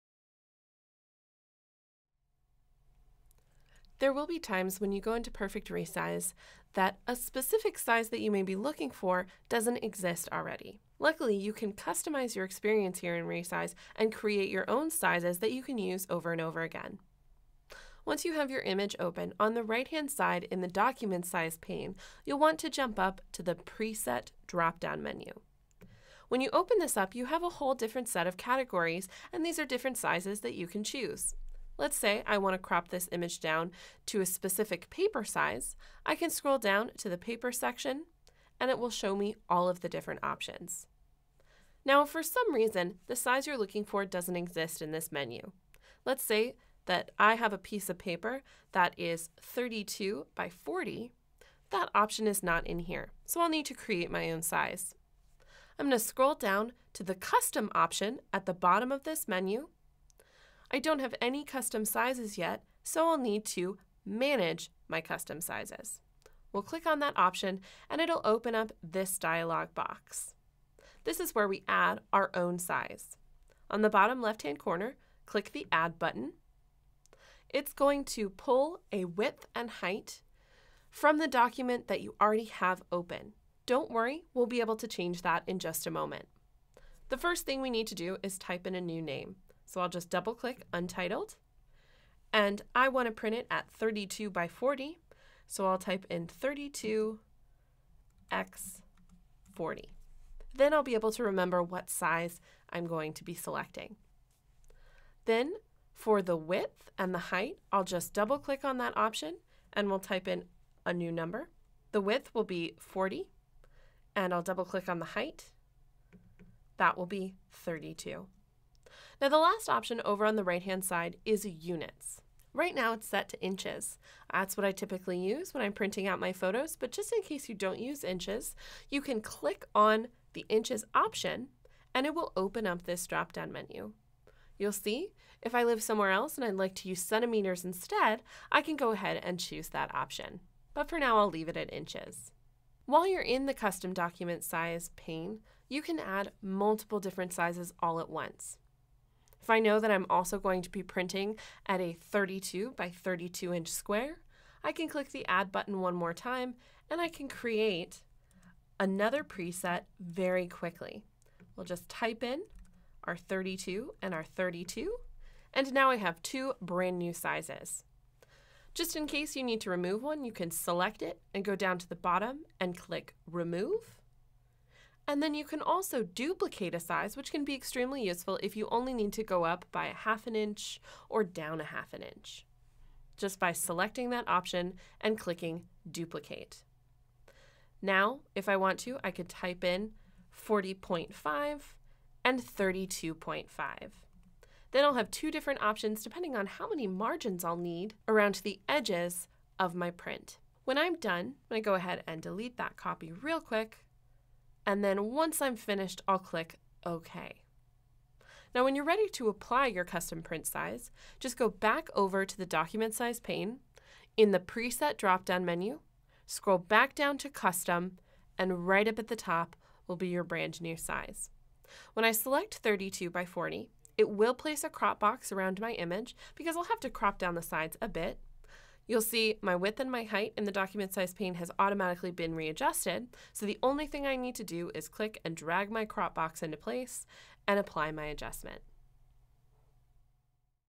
How to Create Custom Print Sizes in Perfect Resize
There are dozens of print size presets available inside Perfect Resize, which can be extremely useful when upsizing your images. However, every once in a while, you may have a specific print size in mind that doesn't exist. Using the Custom Sizes option in Resize, you can adjust your photo's proportions quickly by creating your own size!